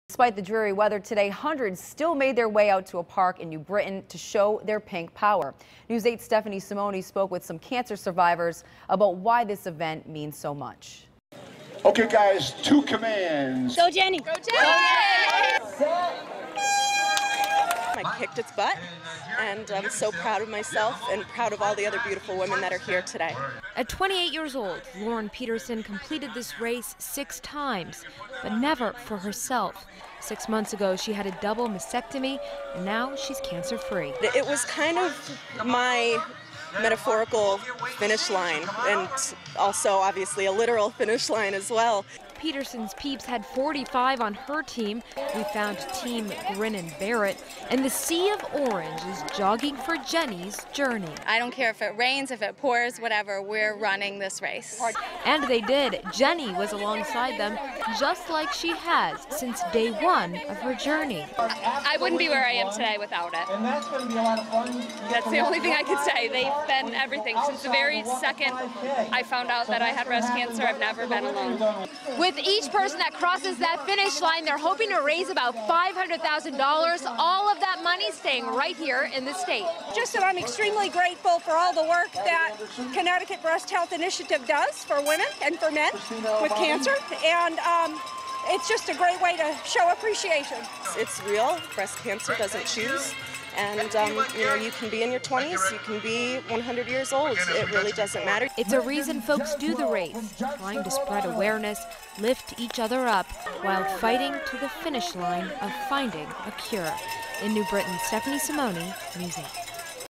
Race in the Park in New Britain benefits breast cancer awareness
News 8's Stephanie Simoni reports on why this event means so much to those involved.